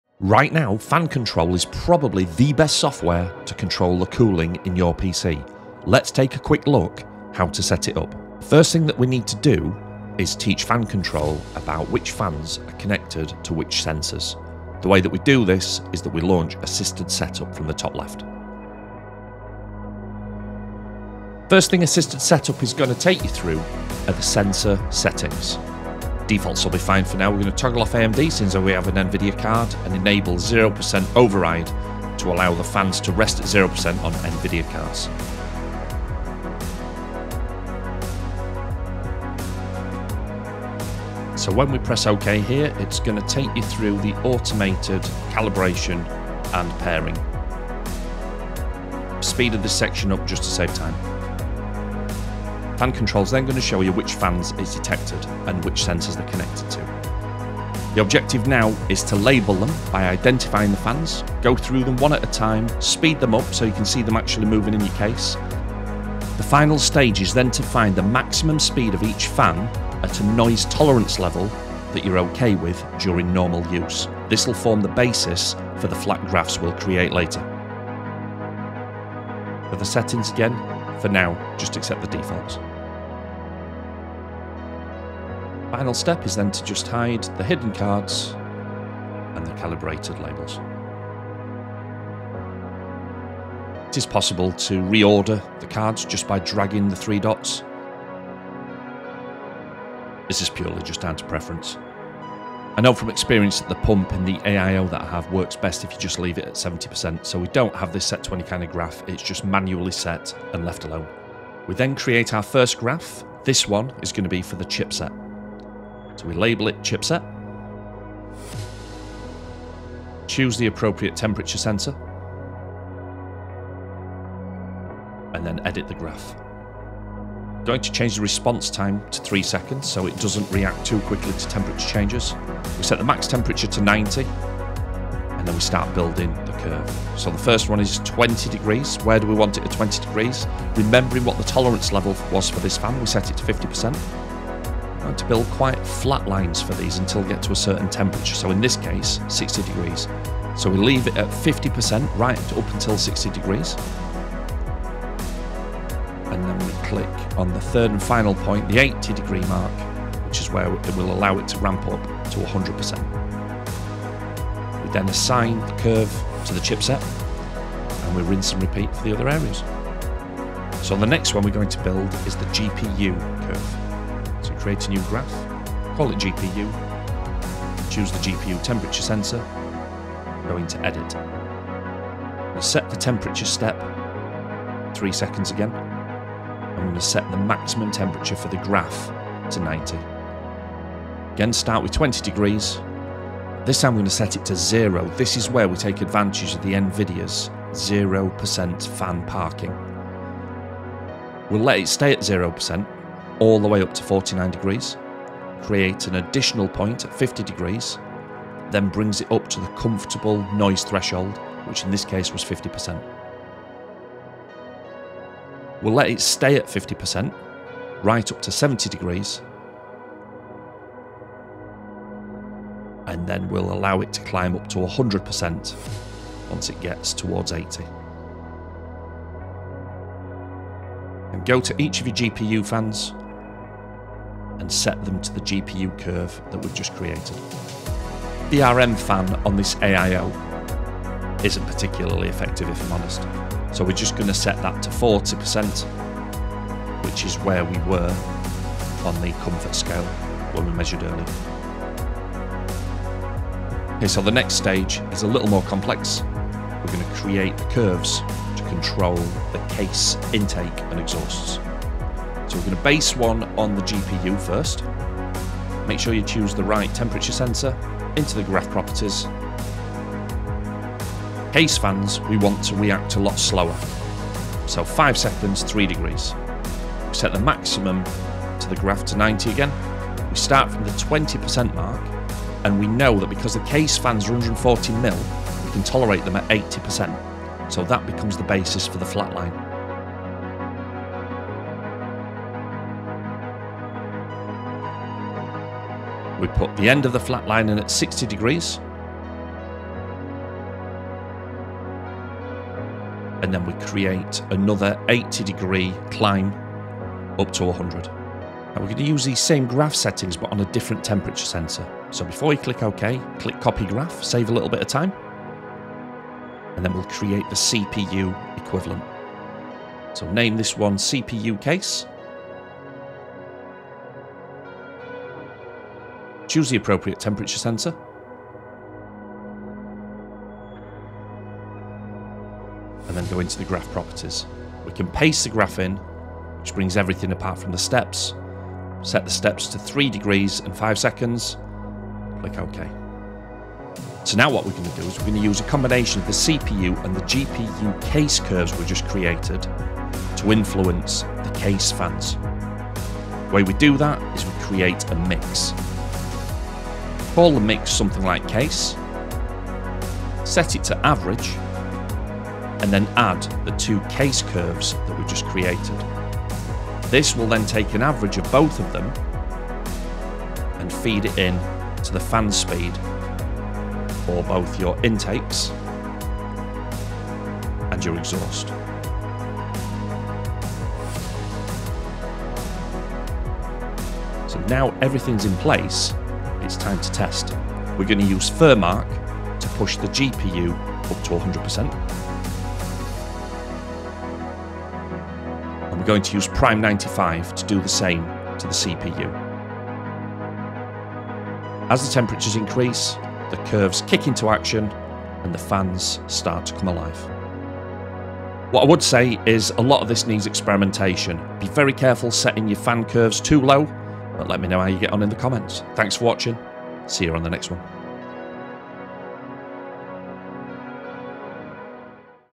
How to Setup FanControl
FanControl is an AMAZING piece of Software for Controlling your PC Fans and Temperatures.  This guide will walk you through setting up FanControl from the start and covers both some basic and more advanced configuration options.

00:00 Introduction
00:08 Assisted Setup
00:52 Identify and Name Fans
01:05 Fan Volume Tolerance
01:24 Customise Layout and View
01:42 Manual AIO Pump Speed
01:51 Single Curve Creation
04:13 Mixed Curve Creation
06:23 Testing using Furmark and Prime95

If you enjoy using the software, please consider supporting the Developer.

ARCTIC Liquid Freezer III 280 (140mm fans)

ARCTIC P14 PWM PST